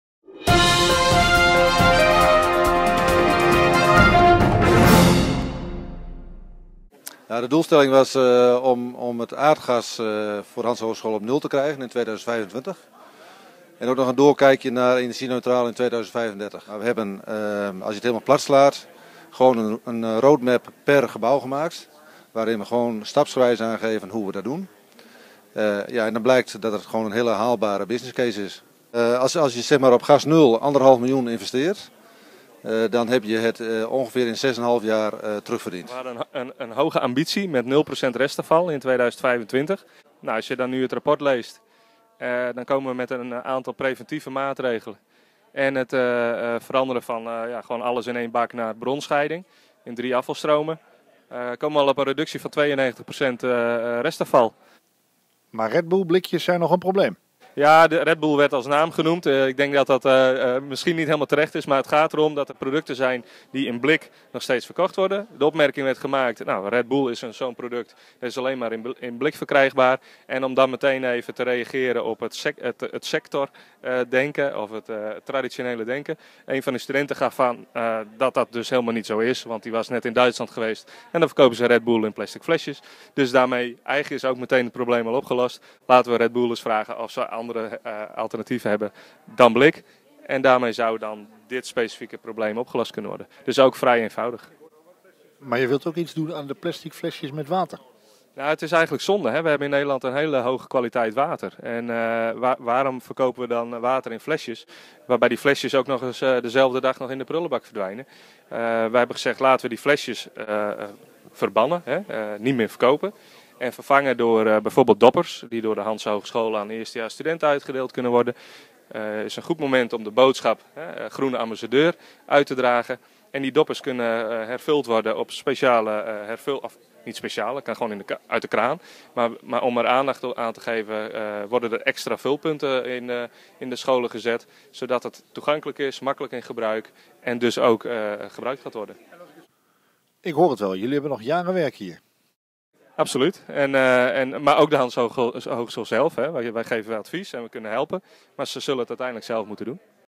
Tjeerd Leistra en Durk Mous lichten bereikte resultaten Green Quest toe
Hoe kan de Hanzehogeschool Groningen in 2025 aardgasvrij zijn en bovendien 92% op restafval besparen?
Tjerk Leistra (ENGIE)  en Durk Mous (SUEZ) leggen het uit.